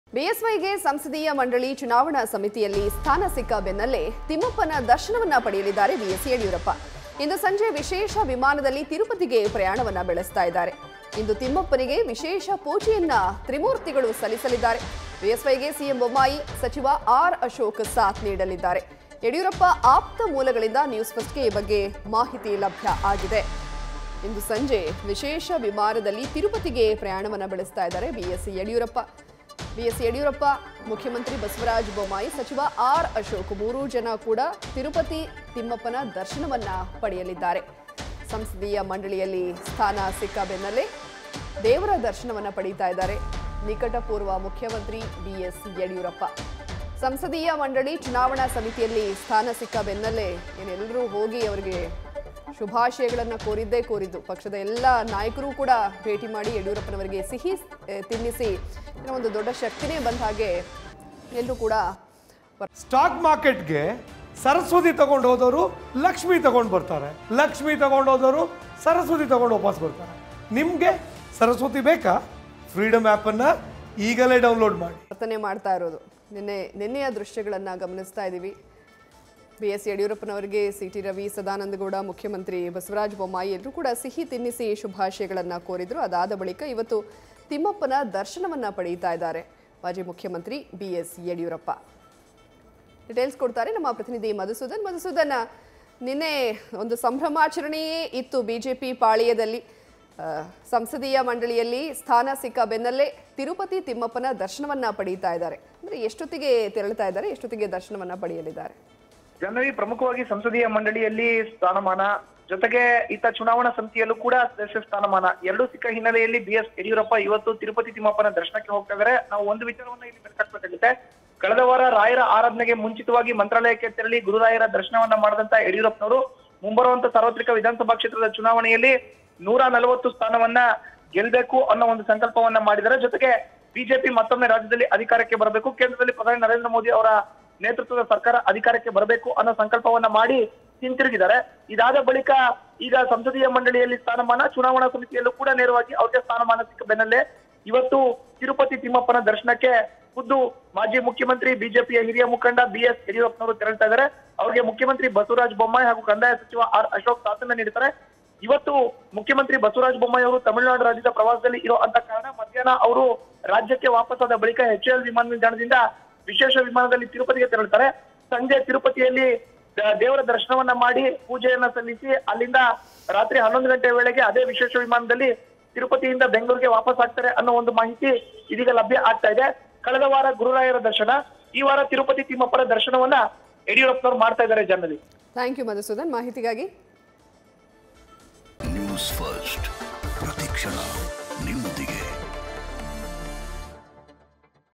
Tirumala : ಇಂದು ತಿಮ್ಮಪ್ಪನಿಗೆ ವಿಶೇಷ ಪೂಜೆ ಸಲ್ಲಿಸಲ್ಲಿದ್ದಾರೆ BSY, CM Basavaraj Bommai & R .ಅಶೋಕ್..
Tirumala : ಇಂದು ತಿಮ್ಮಪ್ಪನಿಗೆ ವಿಶೇಷ ಪೂಜೆ ಸಲ್ಲಿಸಲ್ಲಿದ್ದಾರೆ BS Yadiyurappa, CM Basavaraj Bommai & R .ಅಶೋಕ್..| NewsFirst Kannada